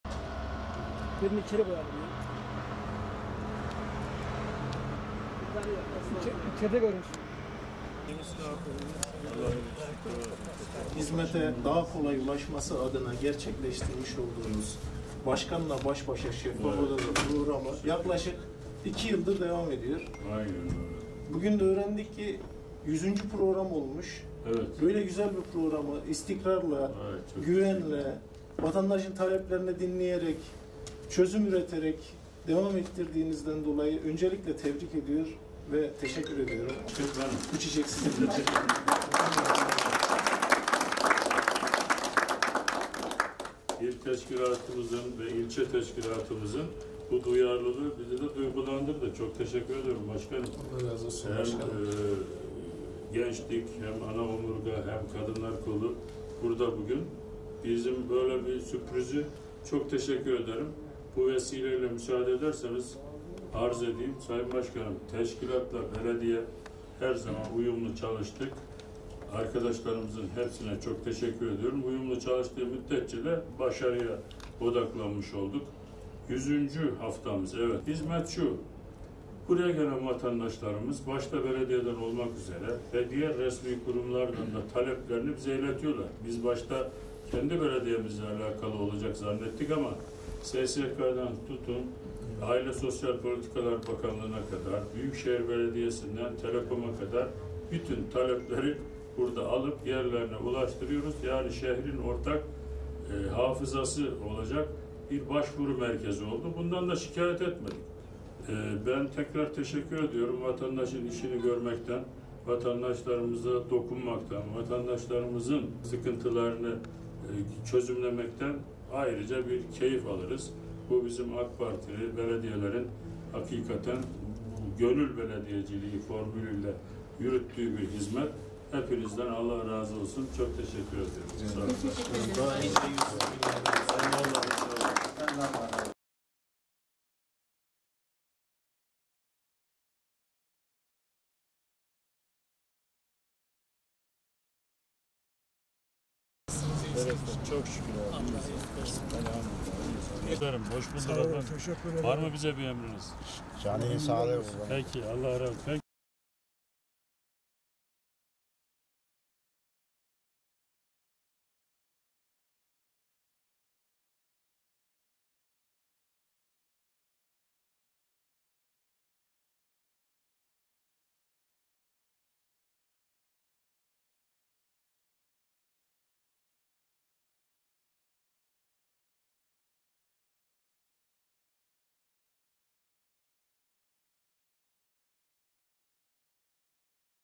100. YILDA ‘ŞEFFAF ODA'DA 100. BULUŞMA’
100. YILDA ‘ŞEFFAF ODA'DA 100. BULUŞMA’
Talas Belediye Başkanı Mustafa Yalçın tarafından 2 yıl önce Türkiye’de bir ilk olarak
başlatılan Şeffaf Odada Başkanla Baş Başa uygulamasında 100. buluşma gerçekleşti.
Başkan Yalçın’ın her hafta çarşamba günü 85 bin nüfusuyla Türkiye’nin en büyük yedinci,
Kayseri’nin ise en büyük mahallesi olan Mevlana’da gerçekleştirdiği uygulama bu hafta da yine
ilgi odağı oldu. Şeffaf Oda’da vatandaşları dinleyen Başkan Yalçın’a AK Parti Talas İlçe Başkanı
Mustafa Kiraz ve yönetim kurulu üyeleri de sürpriz yaptı. Başkan Yalçın’ı tebrik ederek çiçek
takdim eden AK Parti Talas İlçe Başkanı Kiraz, istikrarlı şekilde devam eden uygulamanın büyük
takdir topladığını vurguladı.
Burada kısa bir açıklama yapan Kiraz, “Talaslı hemşehrilerimizin hizmete daha hızlı
ulaşmaları adına gerçekleştirmiş olduğunuz Şeffaf Oda’da Başkanla Baş Başa programının bugün
100’üncüsü yapılıyor. Böyle güzel bir programı istikrarla, vatandaşın taleplerini dinleyerek,
çözüm üreterek devam ettirdiğinizden dolayı tebrik ve teşekkür ediyorum. Allah sizden razı
olsun.” dedi.
“BİZİM İÇİN ÖNEMLİ BİR HİZMET”
Talas Belediye Başkanı Mustafa Yalçın da, teşkilatın bu duyarlılığından dolayı
duygulandığını belirterek, “Ana kademe, gençlik ve kadın kollarının bu davranışı bizim için
sürpriz oldu. Çok teşekkür ederim. Teşkilatla belediye olarak her zaman uyumlu çalıştık.
Arkadaşlarımızın hepsine teşekkür ediyorum. Uyumlu çalıştığımız müddetçe de hep başarıya
odaklandık. 100 hafta önce Şeffaf Oda’yı yaptığımızda insanlara çok inandırıcı gelmedi. Hatta
geçenlerde sayın valimiz buraya geldiğinde ‘Ben bunun geçici bir iş olduğunu düşünmüştüm.
Kalıcı olduğunu düşünmemiştim’ dedi. Ama bu bizim için hakikaten önemli bir hizmetti.” diye
konuştu.
“ŞEHRİN BAŞVURU MERKEZİ”
Başkan Yalçın, Şeffaf Oda’ya gelen vatandaşların taleplerini toplayarak şehrin hafızasını
oluşturan bir merkez olduklarını ifade edip, “Vatandaşlarımız belediyemiz başta olmak üzere
resmi kurumlarla ilgili taleplerini bize iletiyorlar. Biz, başta kendi belediyemizle ilgili talepler
olacağını zannettik ama SGK’dan tutun Aile ve Sosyal Hizmetler Bakanlığı’na, Büyükşehir
Belediyesi’nden Tük Telekom’a kadar bütün talepleri burada toplayıp yerlerine ulaştırıyoruz.
Yani burası şehrin ortak hafızasını oluşturan bir başvuru merkezi oldu. Bundan da şikayetçi
olmadık. Geçen hafta sayın valimizle yapılan bir toplantıda İŞKUR’dan bir görevlinin burada
bulunarak gelen iş taleplerini hemen almalarını istedik. Önümüzdeki günlerde burada onlar da
hazır bulunacaklar.” şeklinde konuştu.
“GÖNÜL BELEDİYECİLİĞİ”
Gönül belediyeciliğinin en güzel örneğini sergilediklerini kaydeden Başkan Yalçın,
“Vatandaşımızın işini görmekten, vatandaşımıza dokunmaktan, sıkıntılarını çözüme
kavuşturmaktan keyif alıyoruz. Bu, bizim AK Partili belediyelerin gönül belediyeciliği formülüyle
yürüttüğü bir hizmet.” ifadelerini kullandı.